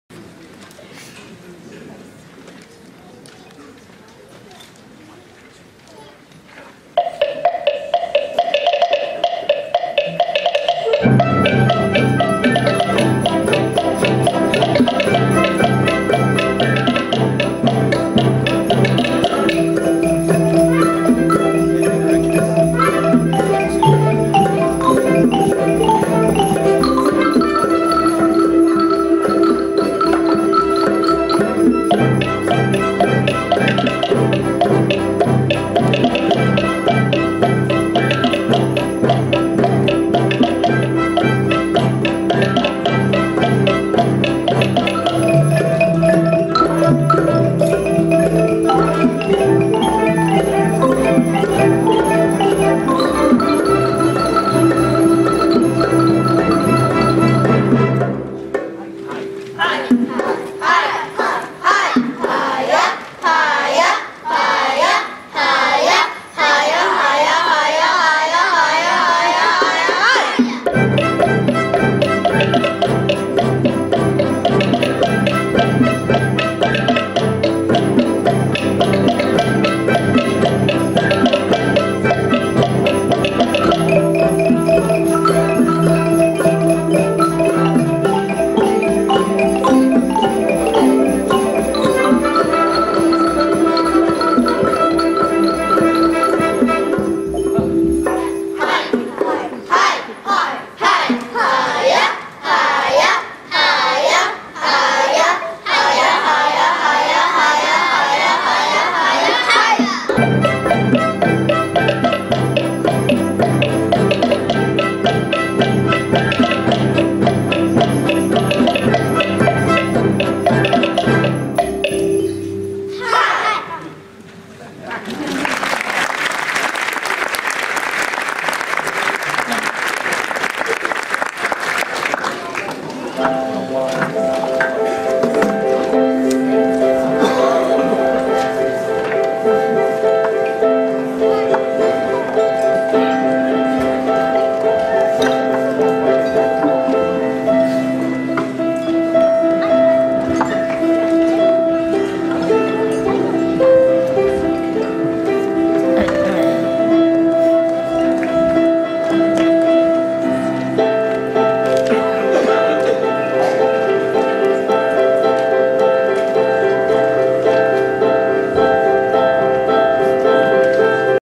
Connor playing xylophone with the Memphis Symphony
Memphis Symphony Orchestra night at Idlewild. Connor playing xylophone.